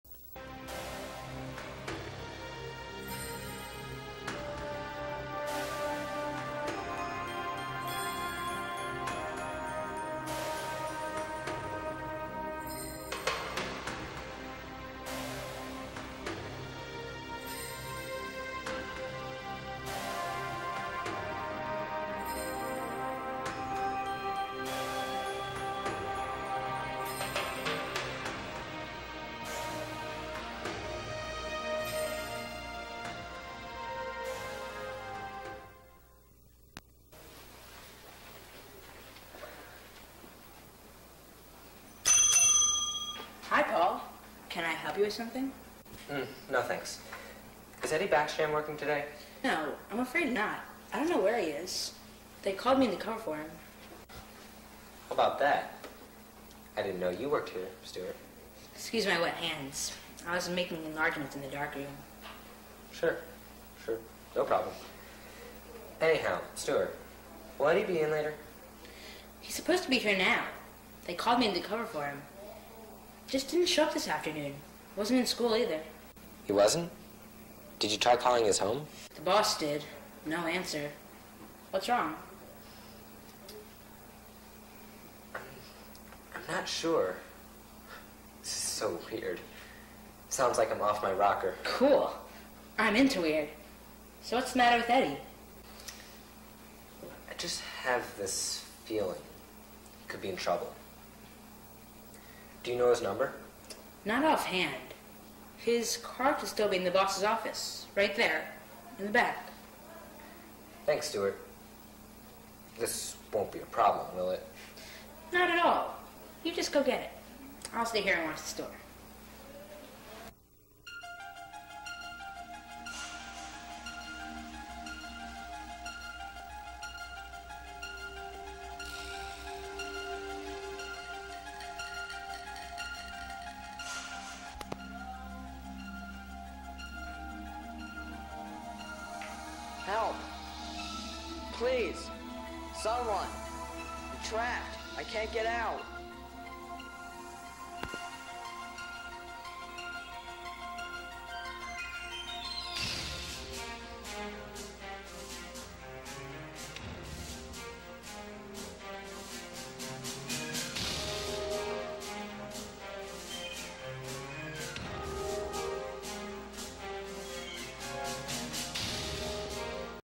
'THE HARVEST' - A Horror Series by CINEKYD, Episode 12 (2004-2005)
Episode Twelve.
Suspense/horror Fall TRACK series.  Cinekyd's last ever completed weekly show.  Enjoy, and please don't be too frightened! ;)

God bless.

And please...

• Our old channel, created by our late founder: @Cinekyd